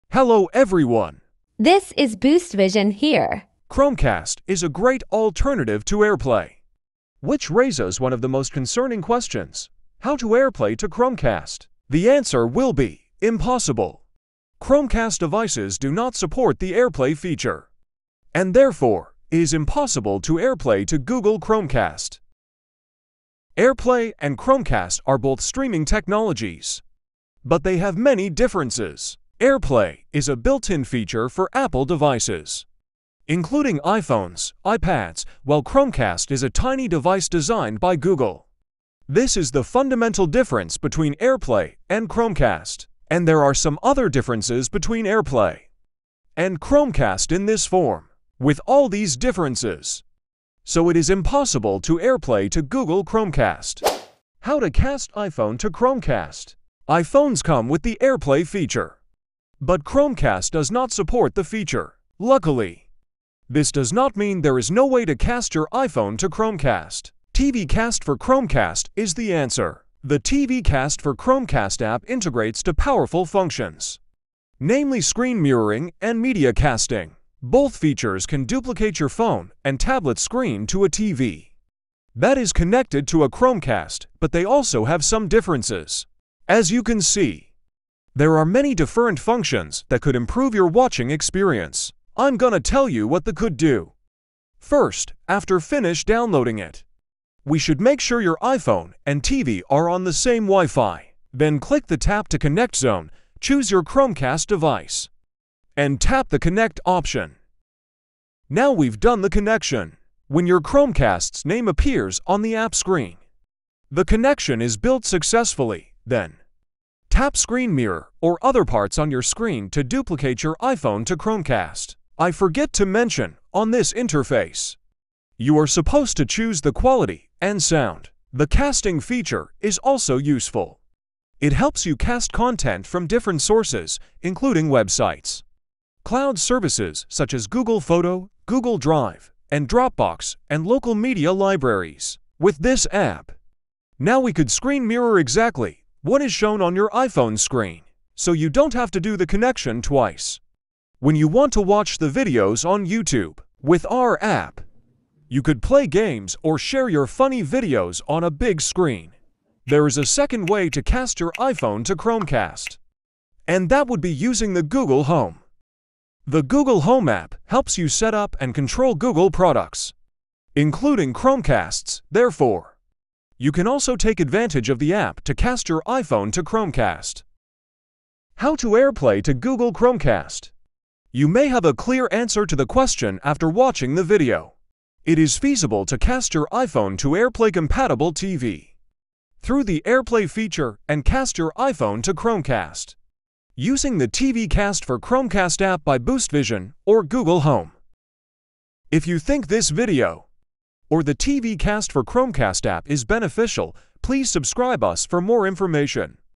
Airplay to Chromecast|Is it possible?[2024 updates]
This video will show you how to use the TV cast screen mirroring app - TV Cast for Chromecast to cast and screen mirror your iPhone to smart TV.

🌟How to screen cast to TV?
TV Cast for Chromecast app
-Tap the device connection button at the top left corner and choose your home TV to connect
-Choose Dropbox to cast media files from it to home TV
-Choose Google Drive to share content from it
-Choose Wed Video to enjoy the videos from the web on the big screen
-Click Videos to cast them from your local library to TV
-Click Photos to cast your photos to larger screen
-Click Music to share your music library with friends & families